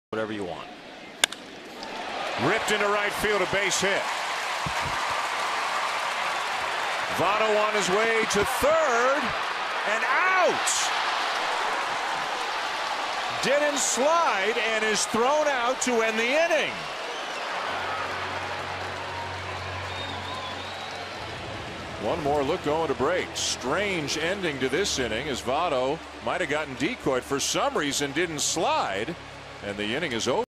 2013/06/10 Beltran's great throw
STL@CIN: Beltran's strong throw nabs Votto at third